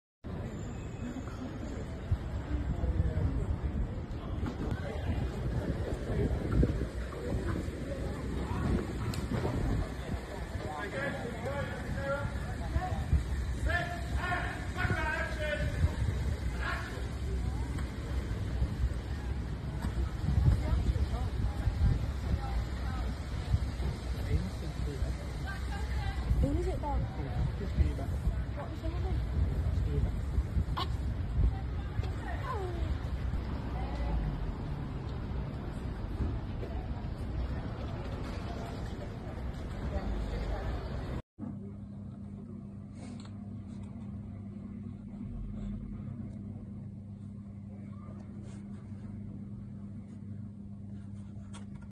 Luke Evans and Julee Cerda were spotted filming on Thomas Street for 5lbs of Pressure.
Luke Evans and Julee Cerda were spotted filming on Thomas Street for "5lbs of Pressure." Manchester, England

Trying to start over, Adam (Evans) searches for redemption as he returns to his old neighborhood to put to rest his demons while the brother of the man he killed seeks revenge.